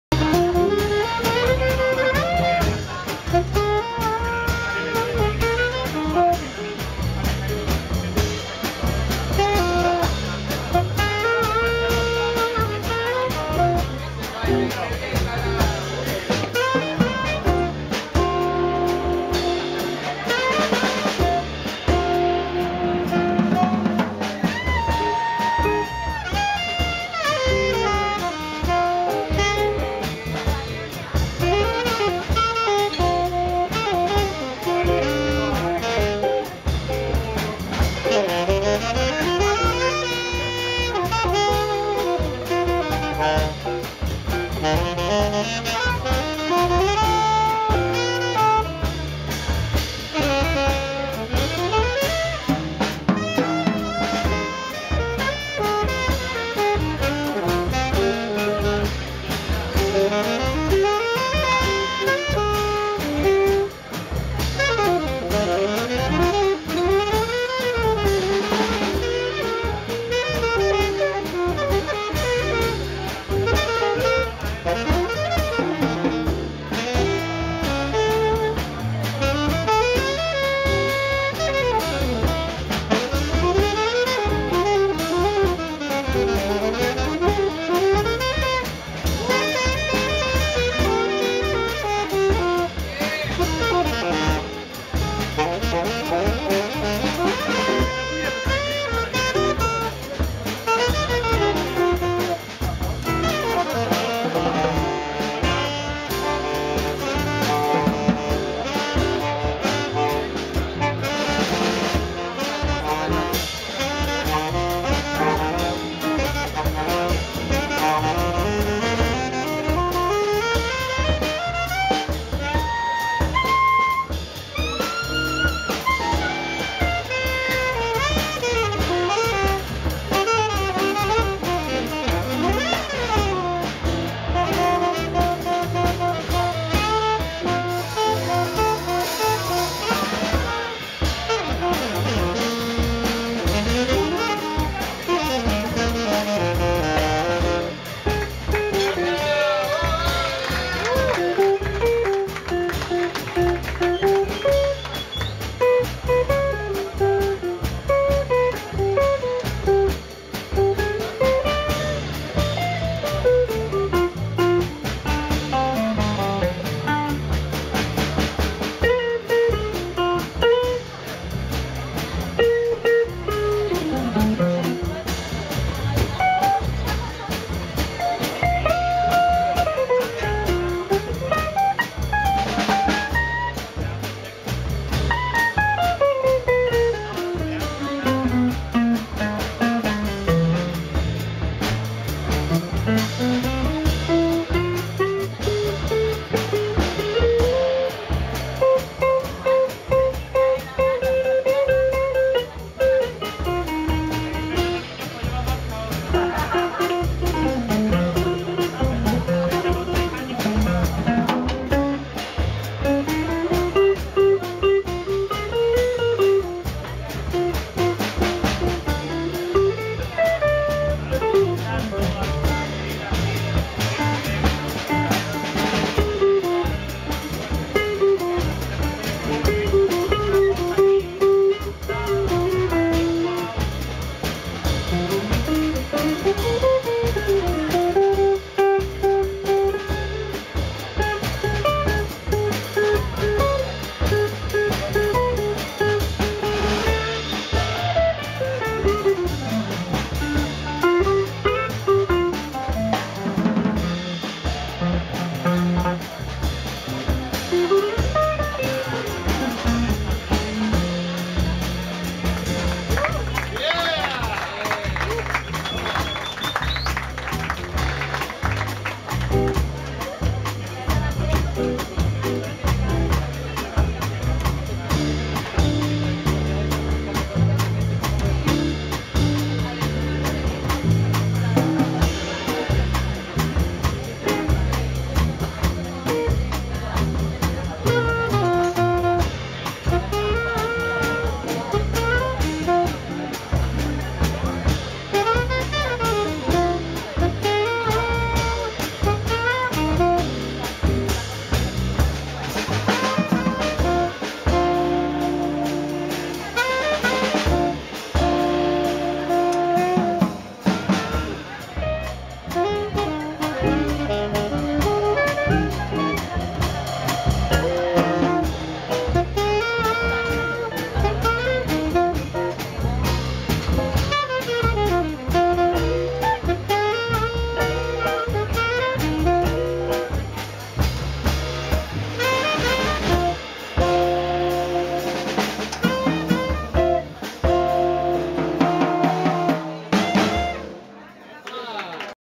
Jam Session - Javier Barral @ gramola
Jam Session
Festival Jazz e Blues de Pontevedra
Javier Barral
gramola - Pontevedra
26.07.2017